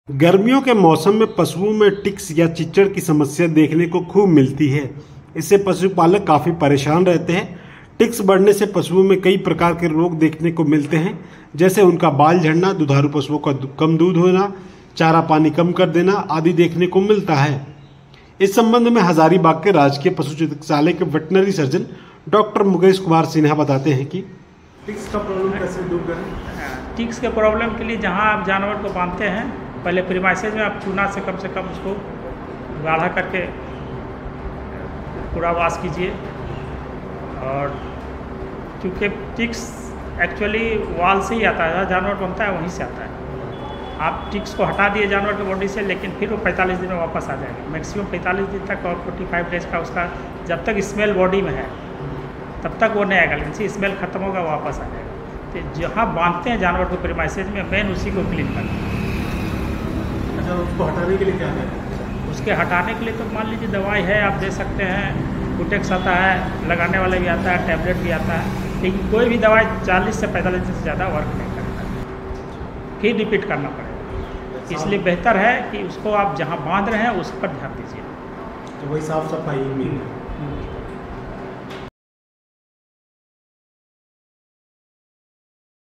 Animal Care: पशुओं में दूर करनी है टिक्स की समस्या, इस चीज का करें इस्तेमाल!
पशु में टिक्स की समस्या बेहद आम है. गर्मी के मौसम की शुरुआत के साथ ही आधिकांश पशुओं में टिक्स देखने को मिल जाते हैं. जैसे-जैसे पशुओं में इन परजीवी का प्रकोप बढ़ता है वैसे-वैसे इससे पशु स्ट्रेस में चले जाते हैं. चारा पानी कम कर देते है. अगर समस्या अधिक दिनों तक रही तो पशु कमजोर हो जाते हैं.

The problem of ticks is very common in animals. With the onset of summer season, ticks are seen in most of the animals. As the incidence of these parasites increases in animals, the animals become stressed. Fodder reduces water. If the problem persists for a long time, the animals become weak.